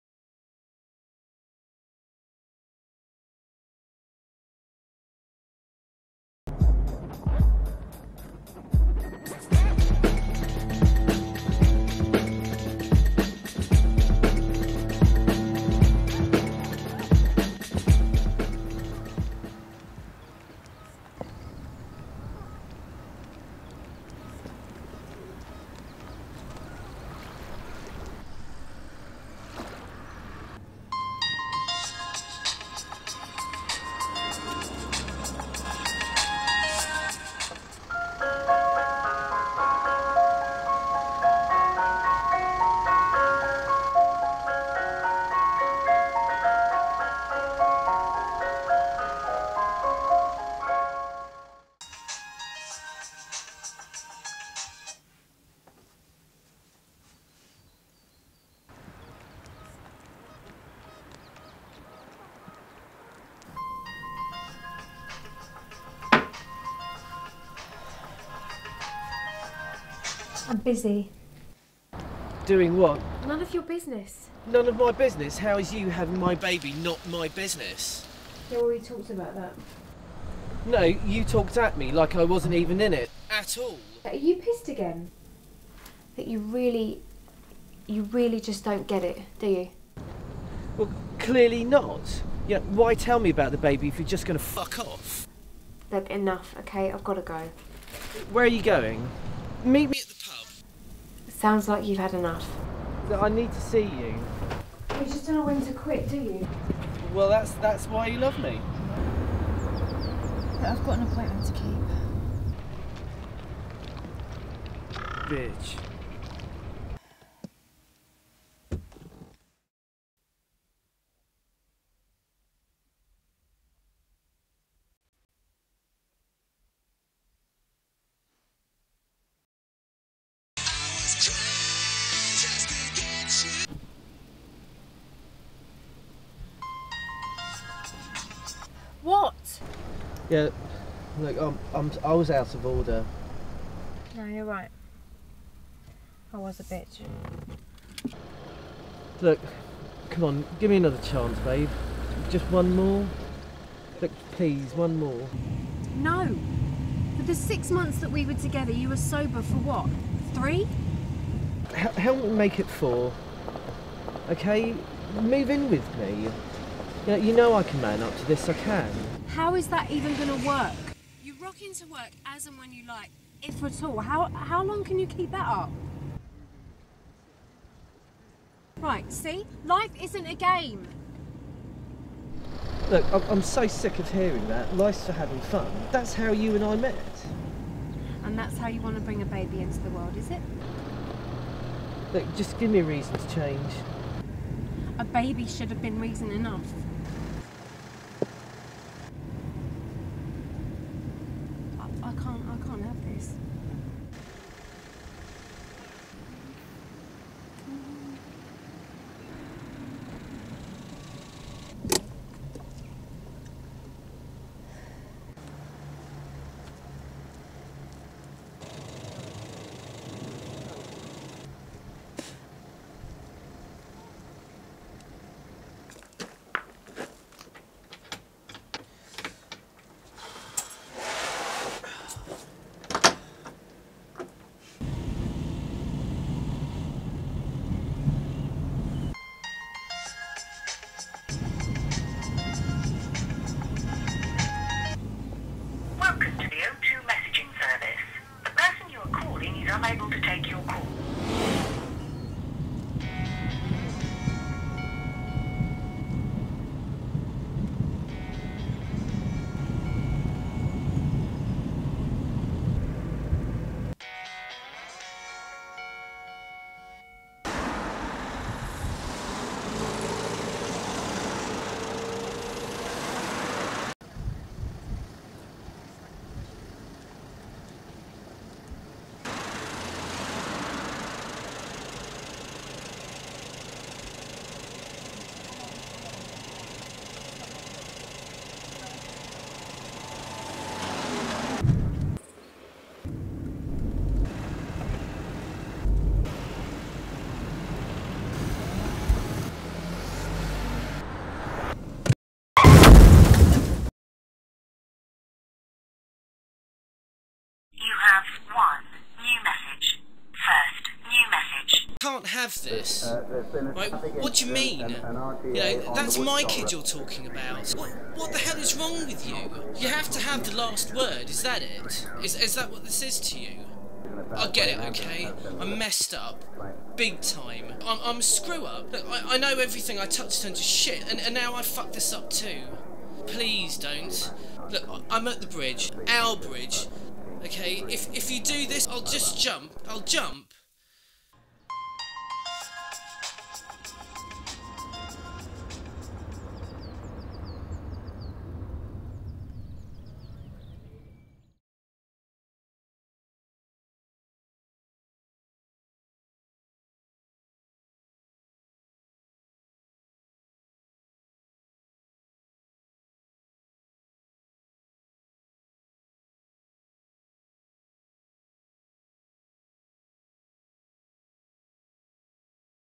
the last word, a student short film
this was my our submission for the end of our first year of uni. would love some feedback.

i know it has flaws but i hope that the story comes across and we were able to paper over the cracks well enough for this to still be enjoyable. it was filled in richmond, kingston, seen and ham during a hot april weekend in 2011. its in black and white because no matter what we tried it was just too bright and sunny in full colour its almost whited out.